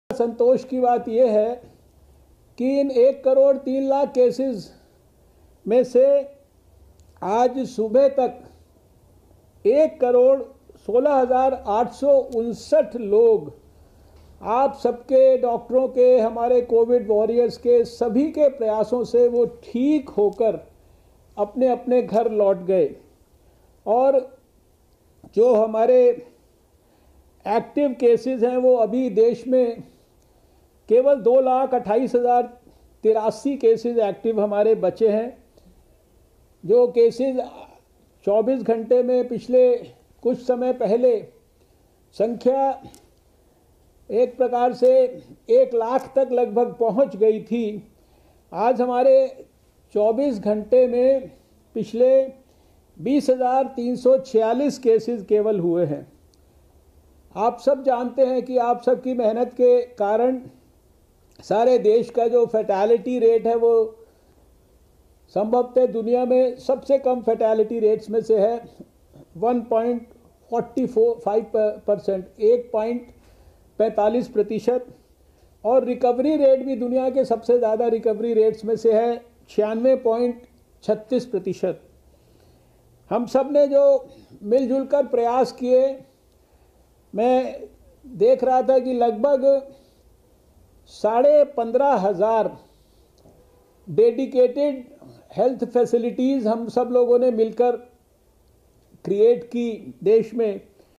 Dr Harsh Vardhan holds meets with state officials over dry run of Covid vaccination
Courtesy: DD